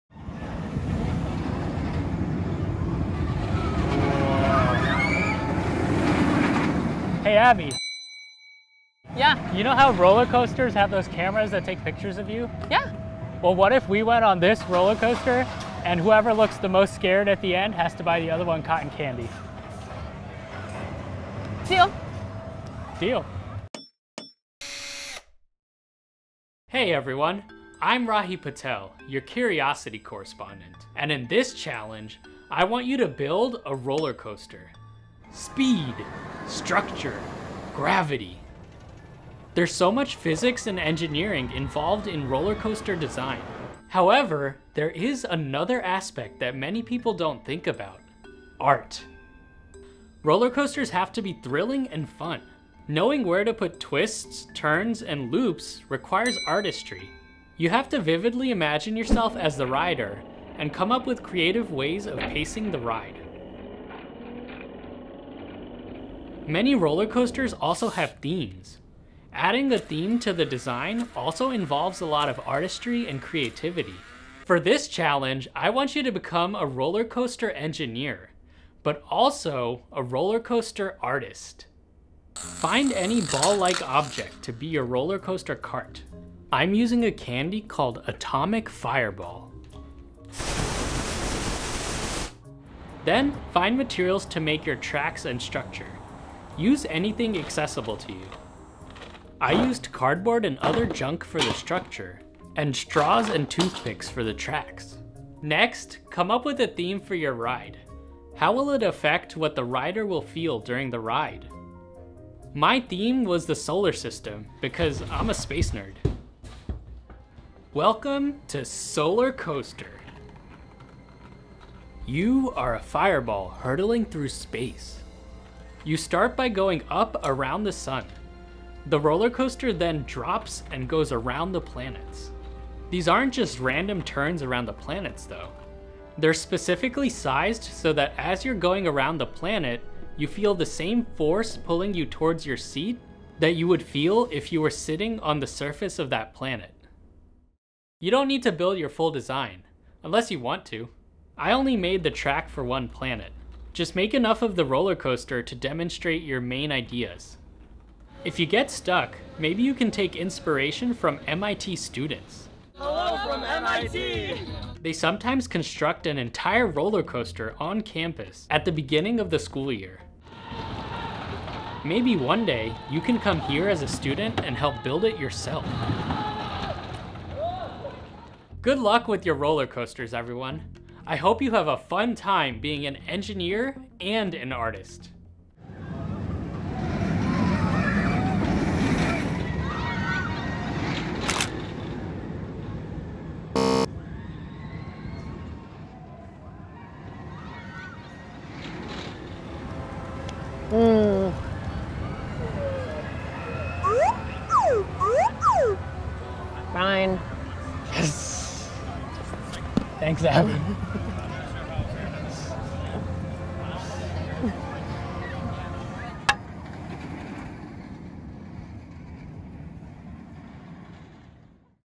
MIT Home Labs: Solar Coaster
Calling all adrenaline junkies! Put your creativity and engineering skills to test when you design and build your own roller coaster! MIT Curiosity Correspondent Rahi learns roller coaster design requires just as much artistry as it does engineering in the latest MIT Home Labs. Can you top Solar Coaster?